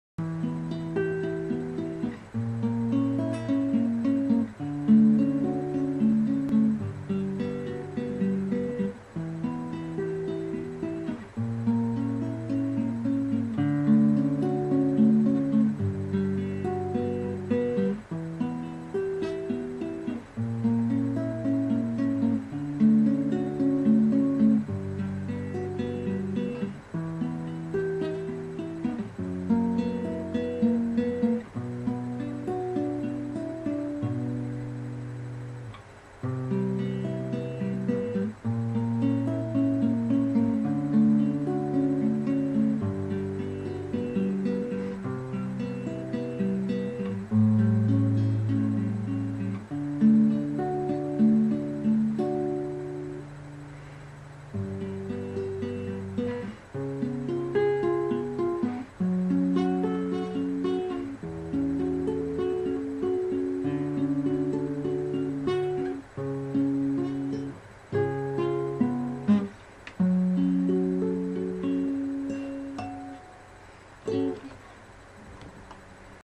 late night relaxation on classical guitar
volumes a bit low. nothing special just something i started playing at 4am and decided to record